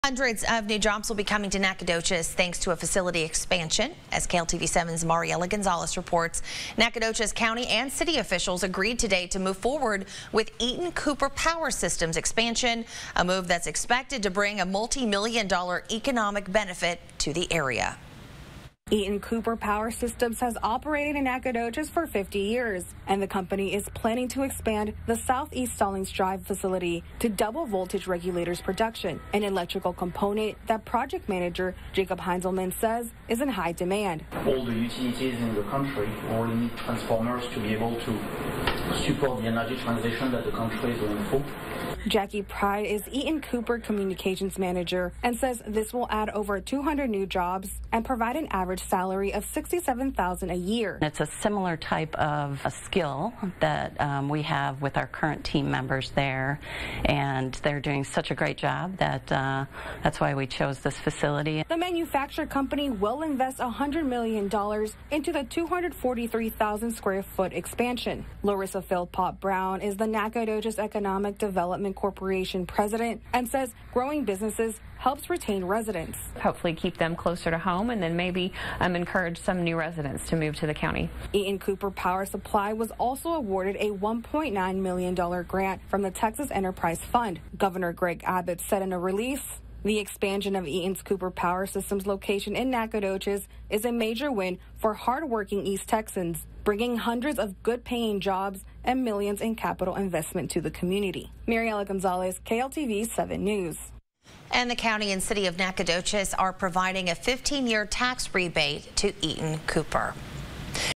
EATON/Cooper Power Systems expansion to add 218 new jobs in Nacogdoches
The expansion and added jobs will provide an additional $14.7 million a year in payroll.